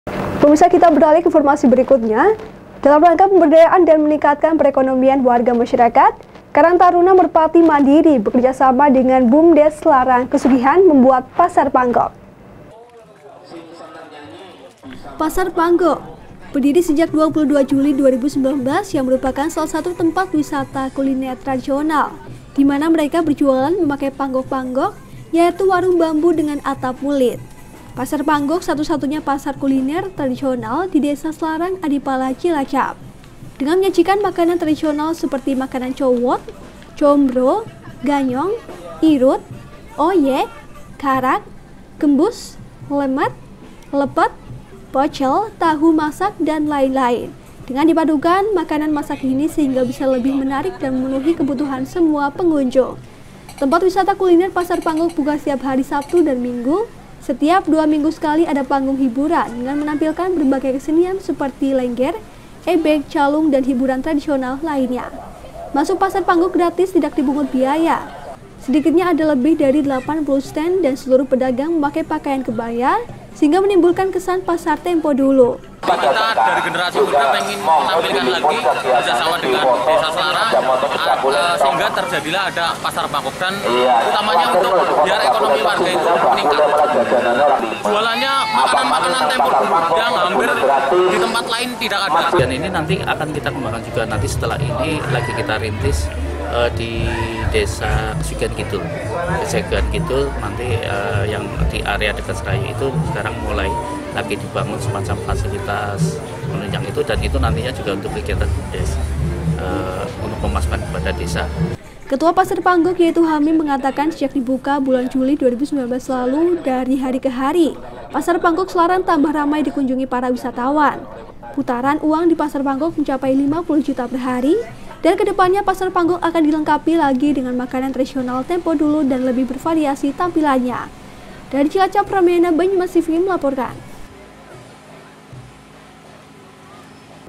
PASAR PANGGOK WISATA KULINER
Dalam rangka pemberdayaan dan meningkatkan perekonomian warga masyarakat, Karang Taruna Mandiri bekerjasama dngan Bumed Slarang Kesugihan Cilacap membuat Pasar Panggok.